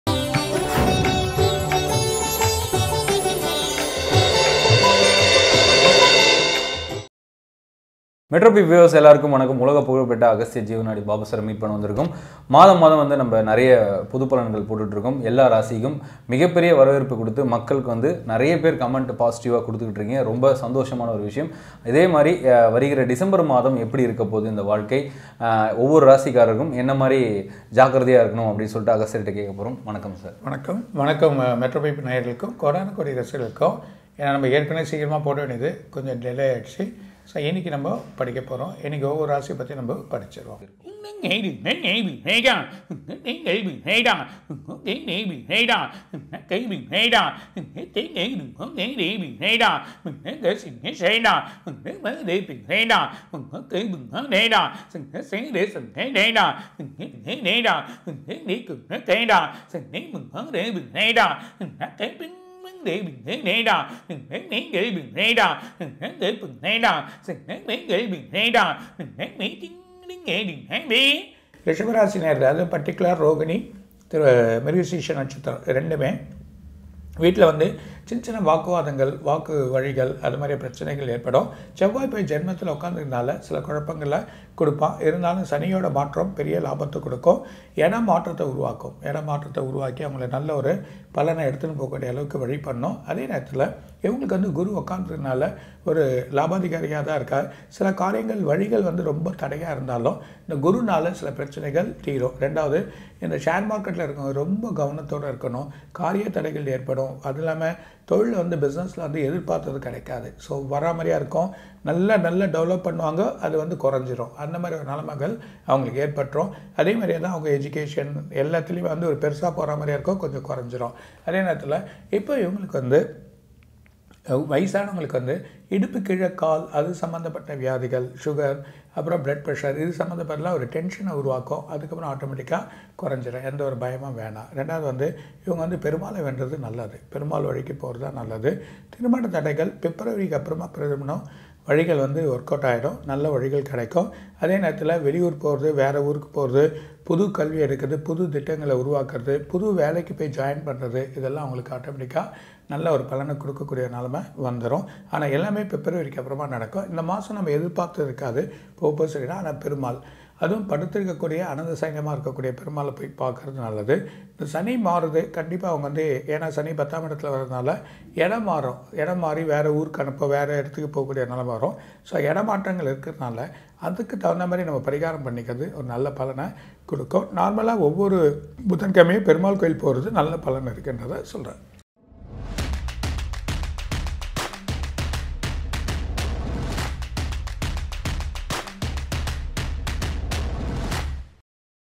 ரிஷபம் ராசி புது வாய்ப்பு லாபம் கிடைக்கும்! "சண்டை காரியத்தடை " இருக்கும் agasthya jeevanadi babu
METROPEEP பக்தி தமிழ் : ரிஷபம் ராசி புது வாய்ப்பு லாபம் கிடைக்கும்  ! சண்டை காரியத்தடை இருக்கும் agasthya jeevanadi babu